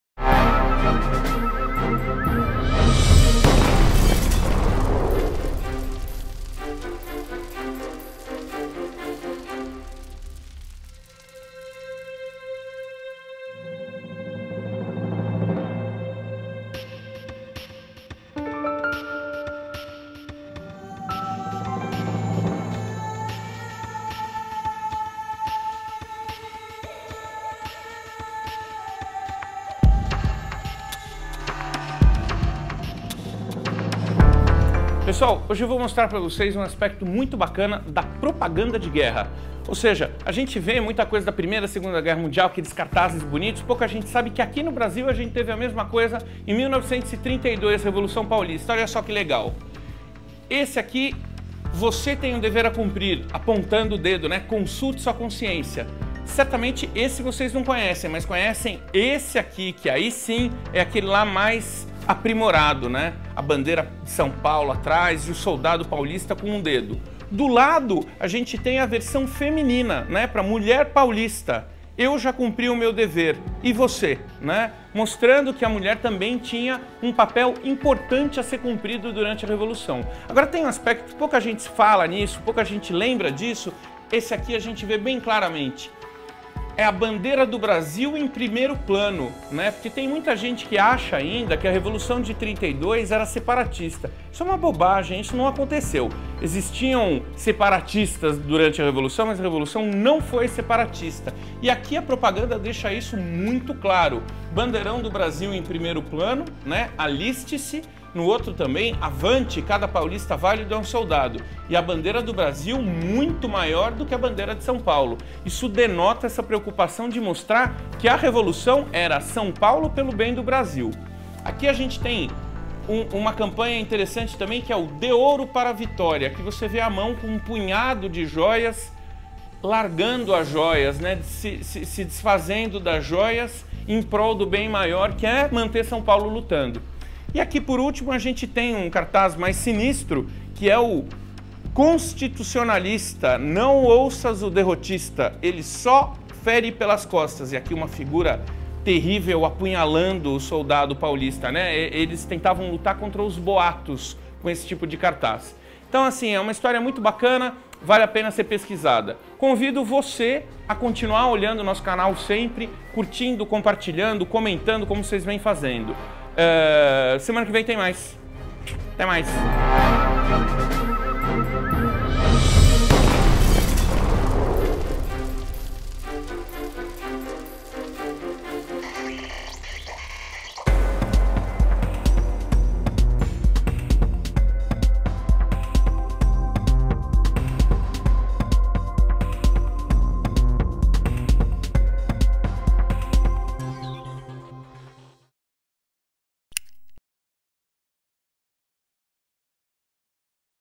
A Propaganda de 1932
Esses cartazes eram criados pelo departamento de propaganda do MMDC (organização civil que coordenava o esforço de guerra paulista). Os artistas são desconhecidos, mas as imagens são célebres e entraram para a história do Brasil.